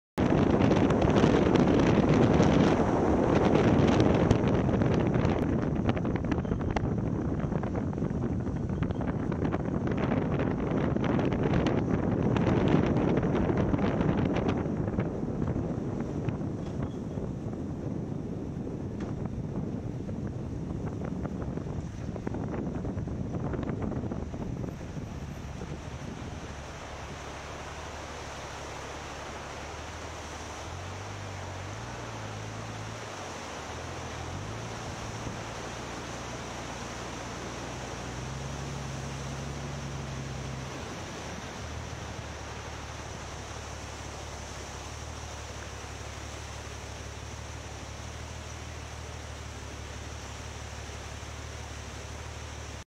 Goldwing water crossing bugtussle over road
Just crossing the Bugtussle flat bridge when flooded on my Goldwing. I just love these water crossings.